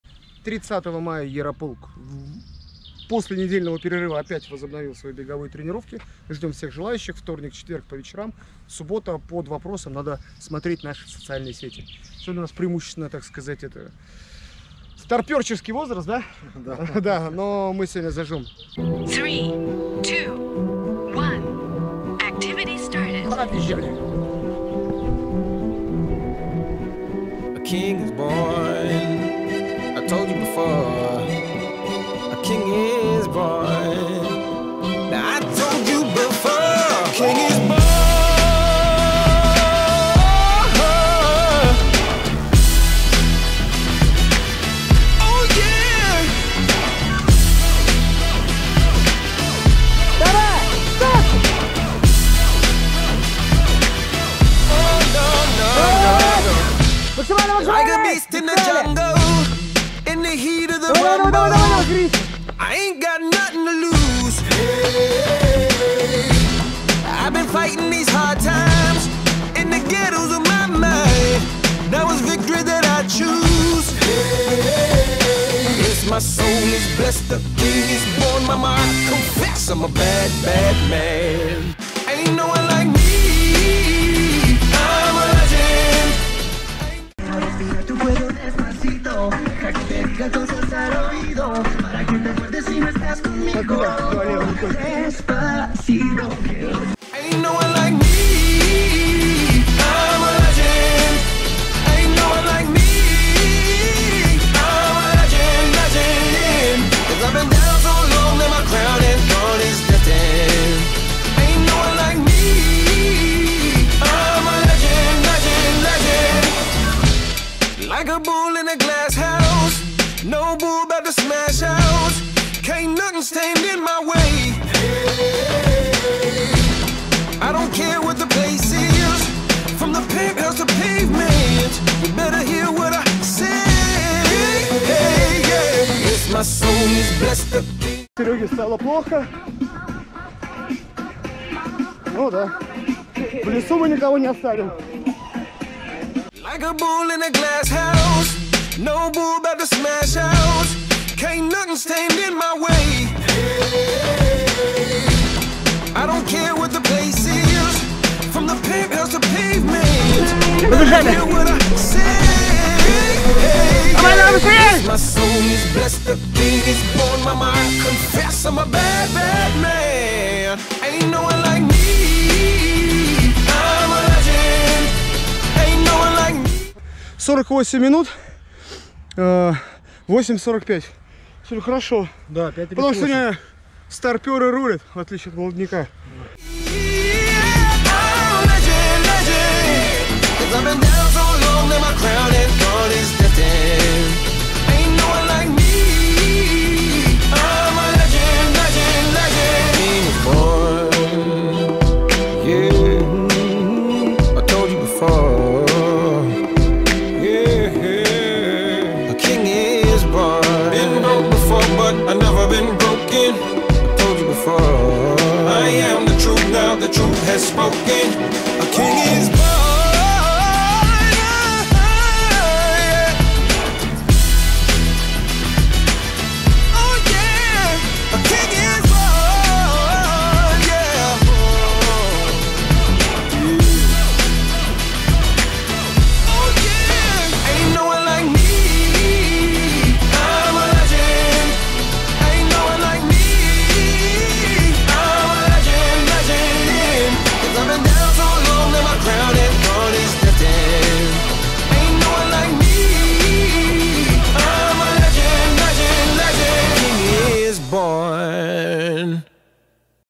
БЕГ в ЛЕСУ-8,5 км / БОКС на СВЕЖЕМ ВОЗДУХЕ / ТРЕНИРОВКА клуба ЯРОПОЛК / 30.05.2023 г.
30 мая 2023 года на спортивной площадке лыжного стадиона г. Красногорск прошла ТРЕТЬЯ тренировка клуба «ЯРОПОЛК» в беговом сезоне 2023 года.
Мы пробежали кросс на 8,5 км за 45 минут. Трасса шла через лес и бег прошёл в одно удовольствие, также были различные силовые задания.
После кросса провели несколько раундов по боксу и закончили тренировку прессом.
2. Клуб в яндекс ДЗЕН
3. Канал клуба в РУТУБЕ